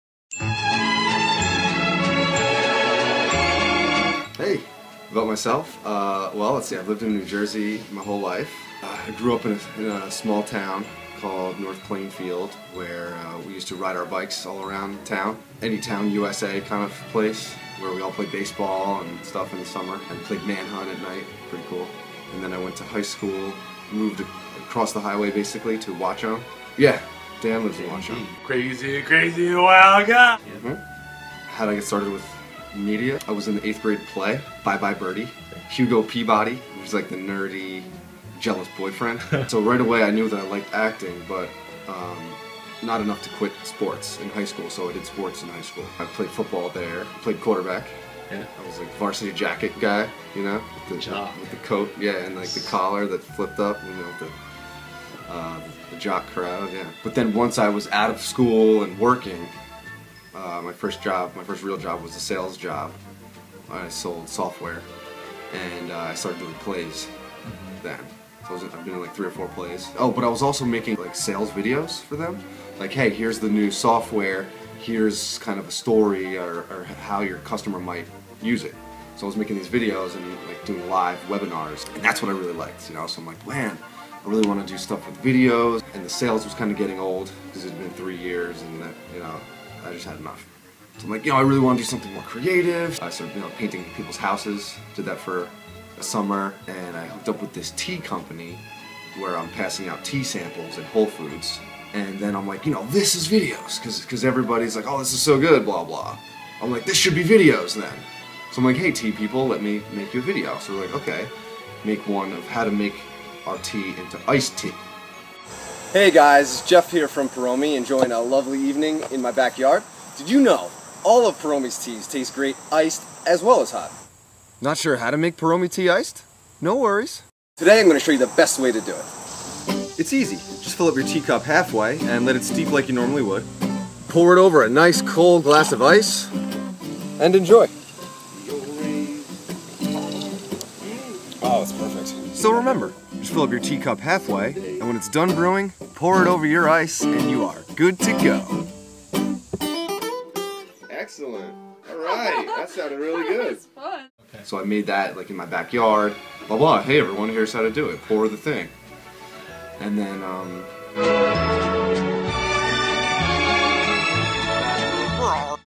Who Do You Think You Are? Ep.1
Get to know PolarTV's host Jeff Bassman in Part 1 of this candid chat with IT's Wes Ignacio.  I didn't really feel like editing this too much so... enjoy anyway!